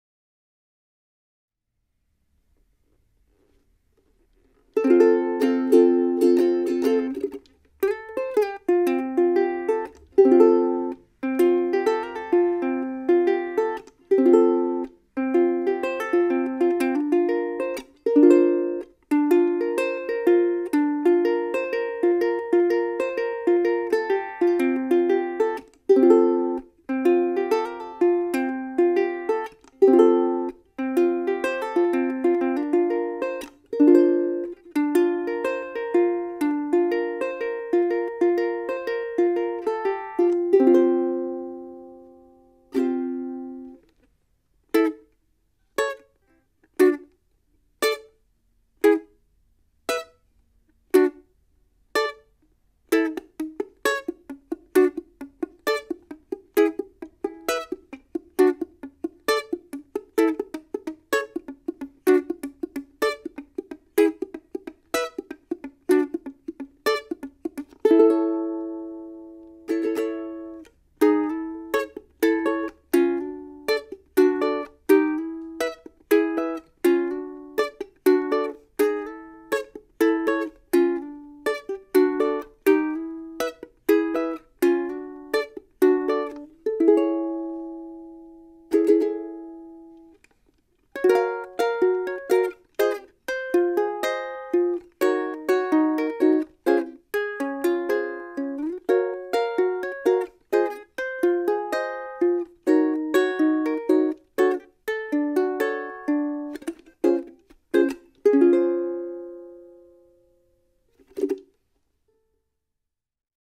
【DaSilva】Soprano Custom #450 ソプラノサイズ
★商品ページ★

カリフォルニアの個人製作家「マイク・ダシルバ氏」によるブランド
”DaSilva”より、カスタムモデルのソプラノ入荷！

ボディトップには杢目の美しいシンカーレッドウッド(杉材)、
サイド・バックにはジリコテ材が用いられ、豪快かつストレートなサウンドながらも包み込むような柔らかい音色が特徴の当モデル。
ボディバックは湾曲した形状の”アーチバック”が採用されており、
ボリューム豊かでウクレレとしては非常に奥行きのあるサウンドです。

ブリッジピンにはべっ甲を使用、指板上のデルタポジションマークにはアバロンを用い、艶のあるグロス塗装で非常に高級感のあるルックスが魅力。

一風変わったユニークなデザインと、弾きやすさ・音色の良さを兼ね備えた
至極のUSA製ビルダーウクレレです。

◆スペック
ブランド：DaSilva(ダシルバ)
品番：Custom Soprano #450
サイズ：ソプラノサイズ
ボディトップ：シンカーレッドウッド
サイド・バック：ジリコテ
ネック：マホガニー
指板・ブリッジ：エボニー(12F/17F)
ポジションマーク：アバロン
サドル＆ナット：牛骨
ペグ：Gotoh UPT-UBB-B
搭載弦：フロロカーボン弦
塗装：ラッカーグロス仕上げ

ハードケース付属

全長：約555mm
弦長：約350mm
ナット幅：約37.5mm

◆DaSilva(ダシルバ)
アメリカ合衆国カリフォルニア州ウクレレ・ビルダー マイク・ダシルバ氏による手工ブランド。世界的なウクレレプレイヤーJames Hillが使用することでも有名ですが、サウンドやルックスなど非常に個性豊かな楽器を製作し、特にプレイヤーの方に人気のウクレレ製作家です。